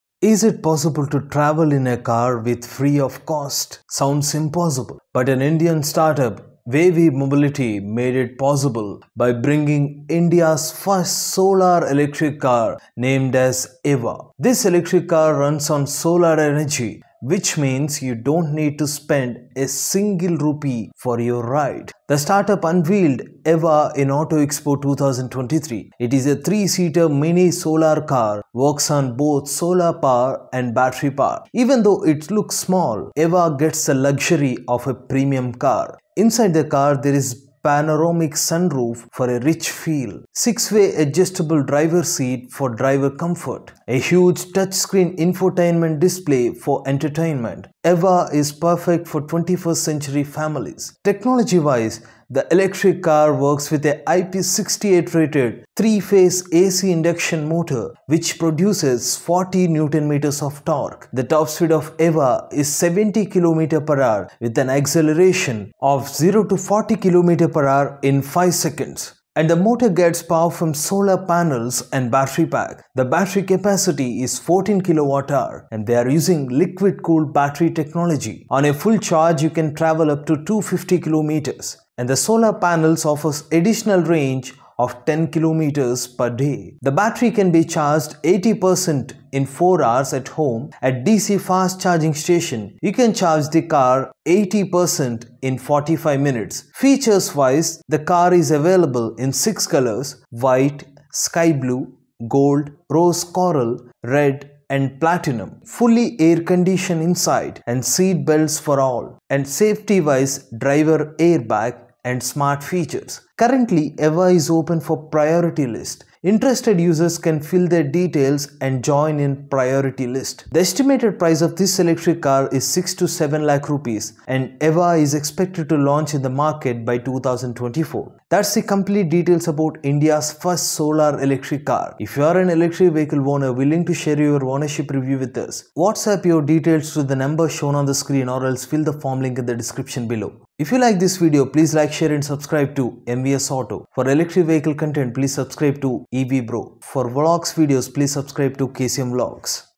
Indias First Solar Electric Car EVA
Indias First Solar electric car EVA complete details.

The electric car works with a ip68 rated 3 phase ac inducstion motor produce 40 nm of torqe
Top speed of  eva is 70 kmph with an acceleration of
0-40 in 5 sec
gets power from solar panels and battery pack

the battery capacity is 14kwh. they are using liquid cooled batterie.
on a full charge you can travel upto 250 km.

 the solar panels offers additional 10 km of range per day.